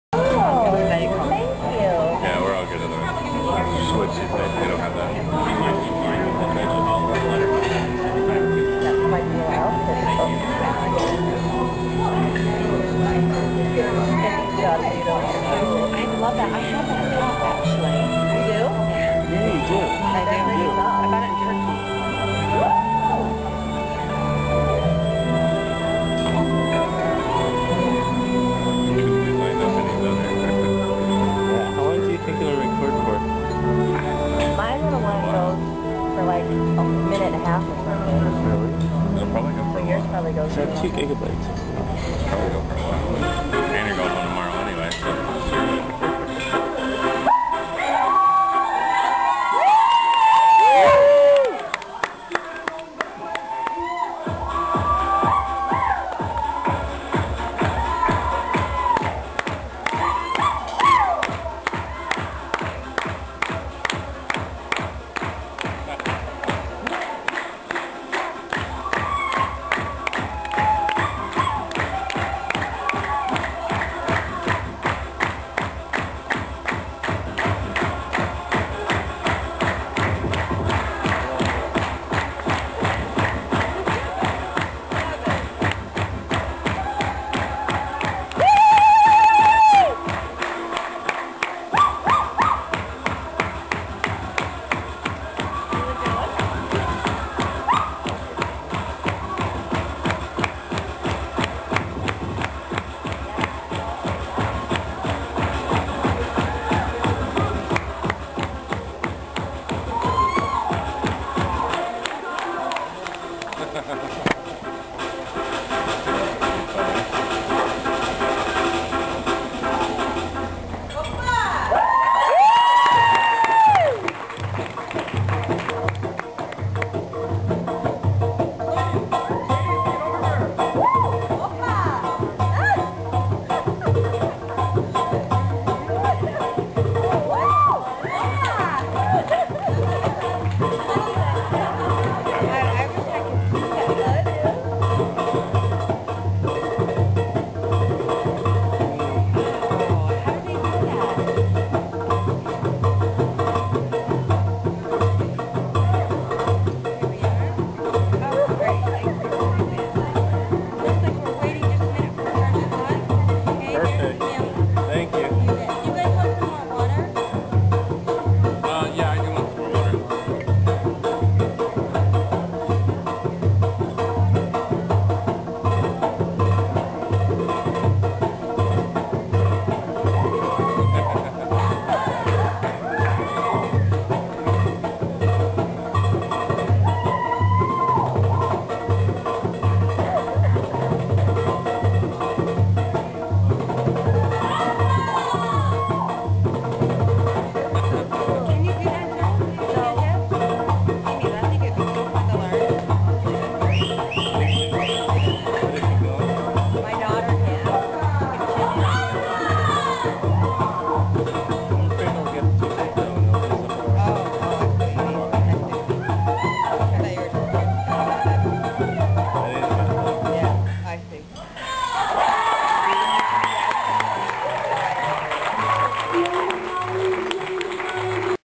SLC Belly Dancer
Belly dancer performs in a Greek Restaurant in Salt Lake City.